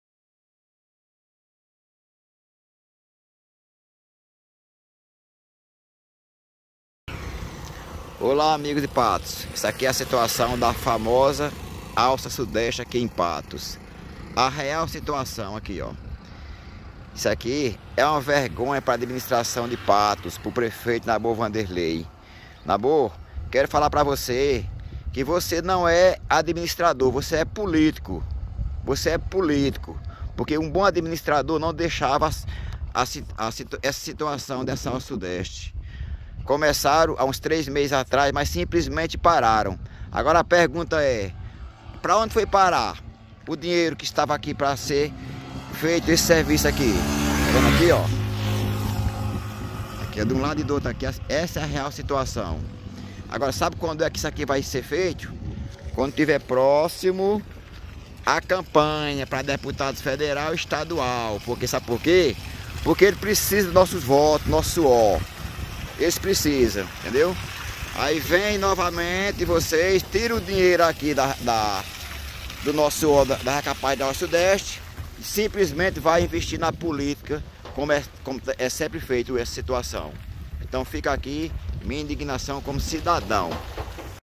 Empresário denuncia descaso na Alça Sudeste, em Patos PB
Na manhã da sexta-feira, dia 04, o empresário Edmilson, mais conhecido por Brother, decidiu fazer um vídeo como forma de protestar em relação ao abandono das obras de recuperação e melhorias do asfalto na Alça Sudeste, em Patos.

Edmilson fez um desabafo e acusou o prefeito Nabor Wanderley (Republicanos) de abandonar de forma proposital a obra. Para o empresário, a obra foi paralisada para ser retomada quando estiver próximo da campanha para deputado estadual e federal.

A Prefeitura Municipal de Patos, através da Secretaria de Infraestrutura do Município, deu início em dezembro de 2021 às obras de recapeamento asfáltico, ampliação e melhoria na Alça Sudeste, porém, a empresa responsável pelos serviços paralisou e não se tem mais nenhuma informação da retomada dos serviços.

A reportagem do Polêmica Patos fez contato com Júnior Bonfim, que é o secretário de Infraestrutura do Município de Patos. O secretário relatou que aguarda resposta de ofício enviado à empresa Engelplan, responsável pela obra de recuperação da Alça Sudeste, para saber o motivo da paralisação dos serviços.